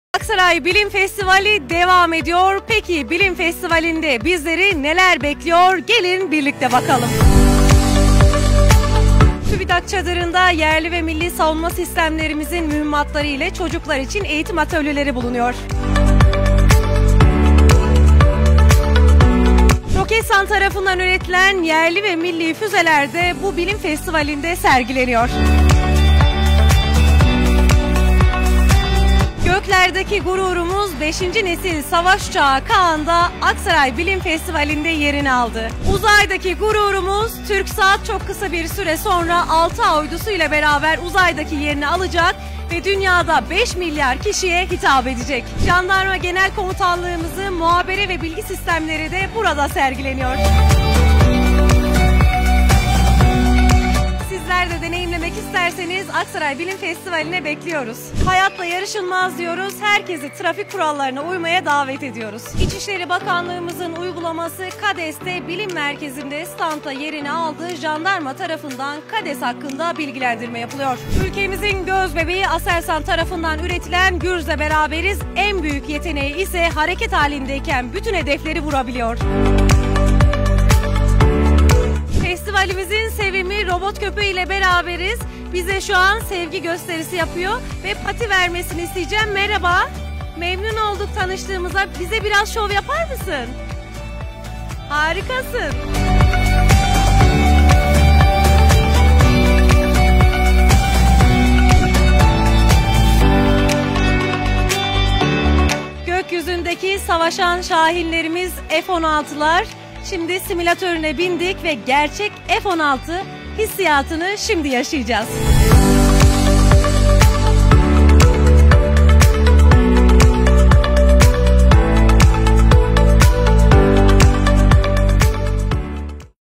❓Bilim Festivalinde Sizleri Neler Bekliyor? 😎
Kades ❤️
F-16 Simulatör 🛩️
Yerli ve Milli Füzeler 🚀
Göz Bebeğimiz Gürz 🧿
Uzaydaki Gözümüz Turksat 🛰️
Yerli ve Milli Savunma Sistemleri 🚀
Göklerdeki Gururumuz 5. Nesil Savaş Uçağımız Kaan 🛩️